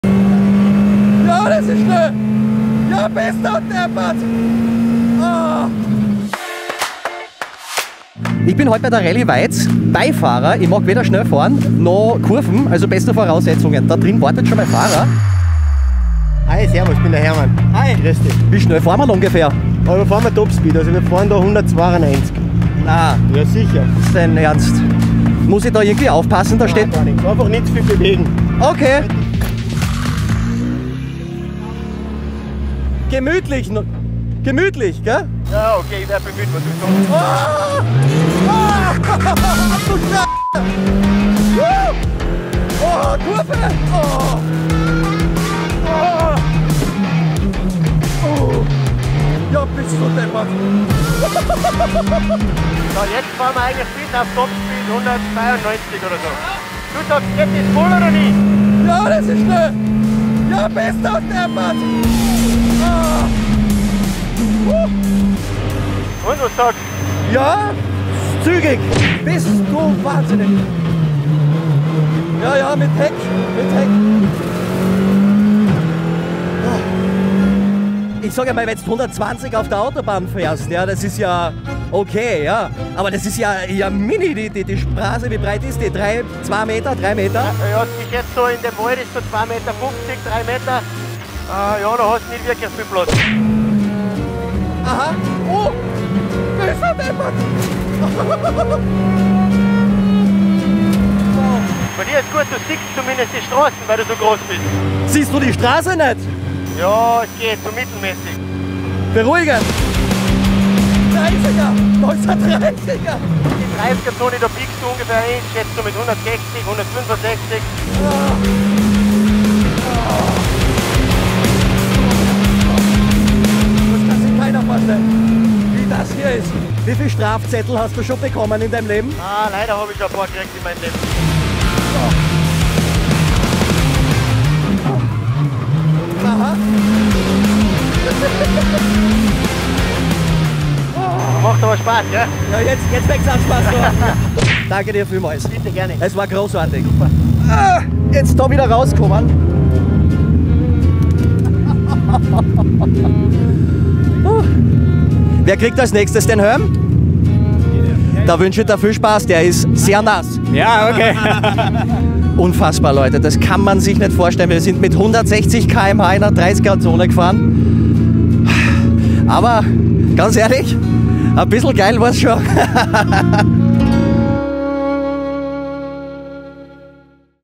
Mit 190km/h über Bergstraße - Muntermacher Markus Dietrich als Beifahrer bei der Rallye Weiz
Antenne Steiermark Muntermacher Markus Dietrich durfte diesmal neben Rallye-Ass Hermann Neubauer am Beifahrersitz platz nehmen. In seinem 300PS starken Skoda sind die beiden mit einer Spitzengeschwindigkeit von 190 km/h bei der Rallye Weiz über die Bergstraßen gerast.